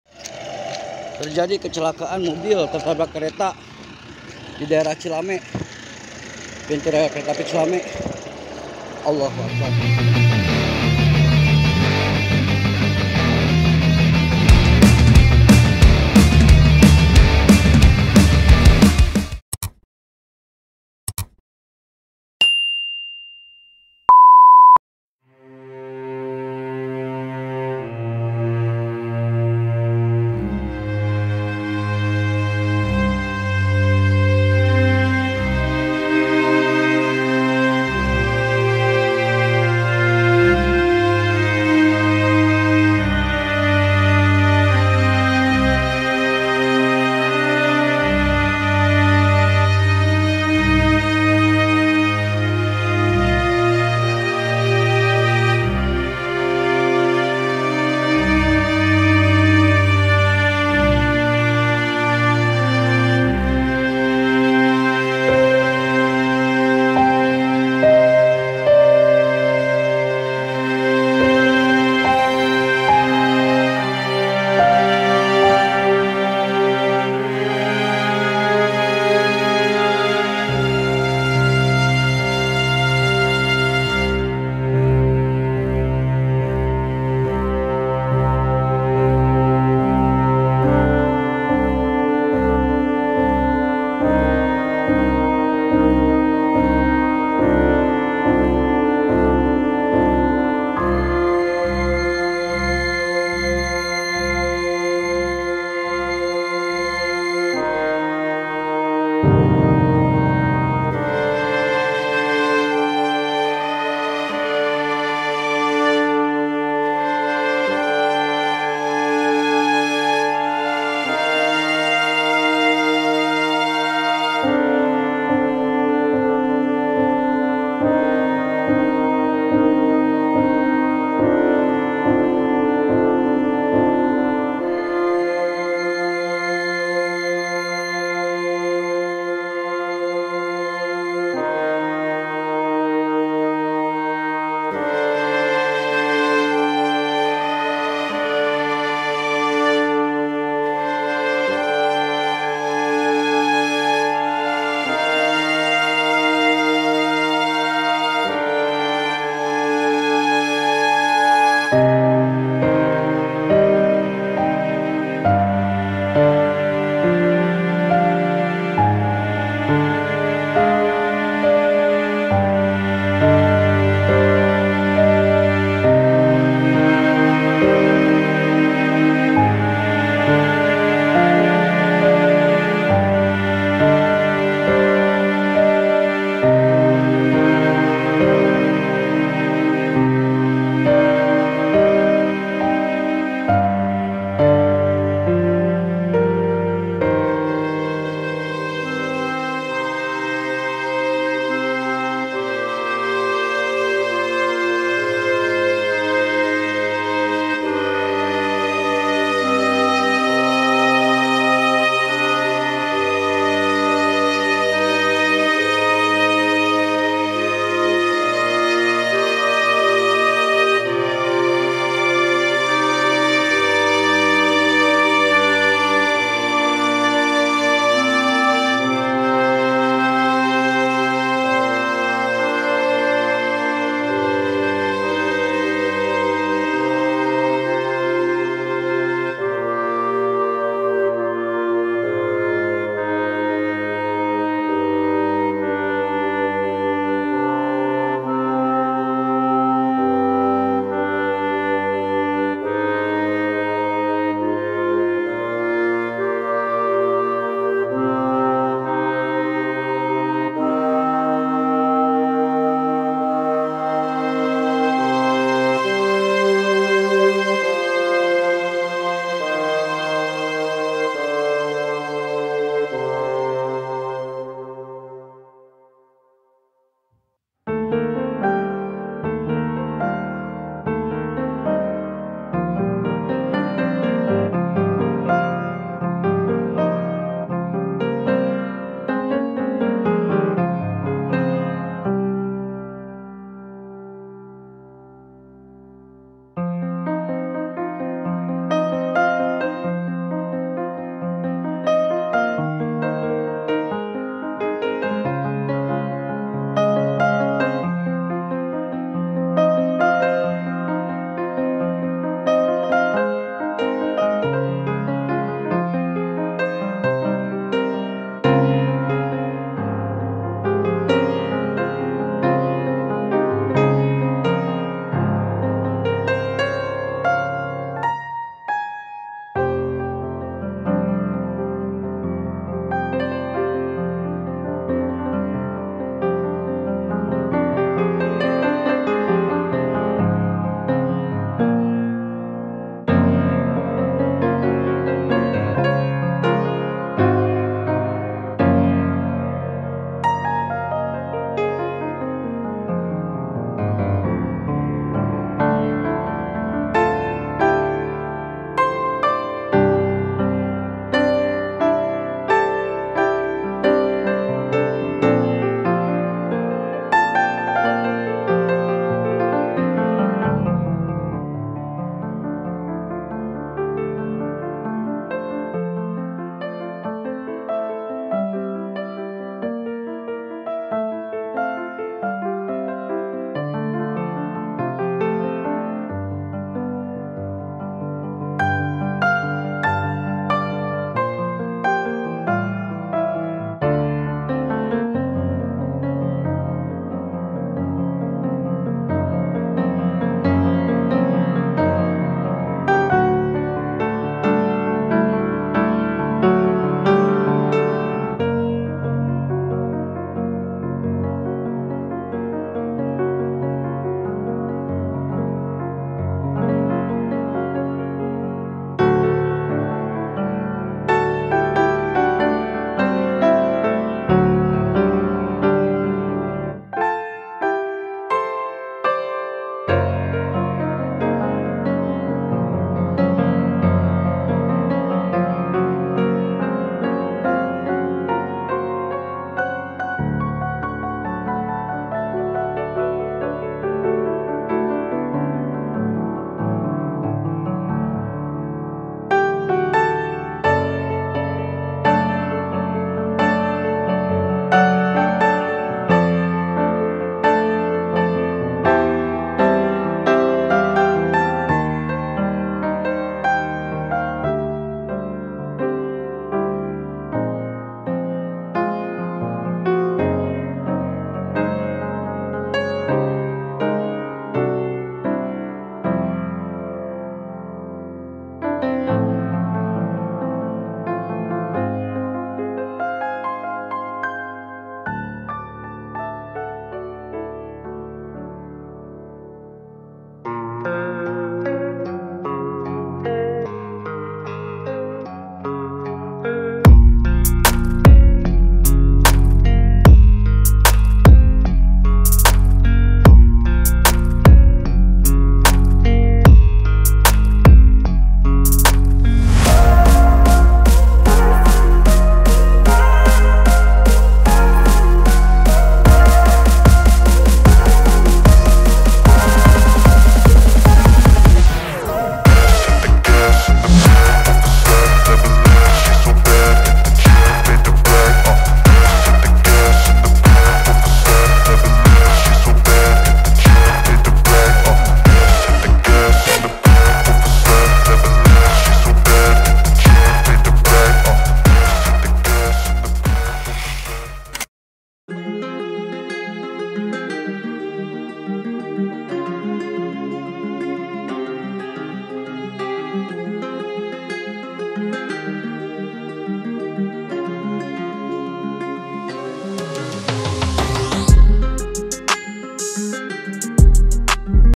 Ngerii‼️Detik-detik Kereta feeder kereta cepat T4br4k Toyota Sigra Di Desa Cilame Bandung Barat
Ngerii‼️Detik-detik Kereta feeder kereta cepat T4br4k Toyota Sigra Di Desa Cilame Bandung Barat,Kamis 14 Desember 2023
Menggambar Kereta Api T4br4k Mobil toyota sigra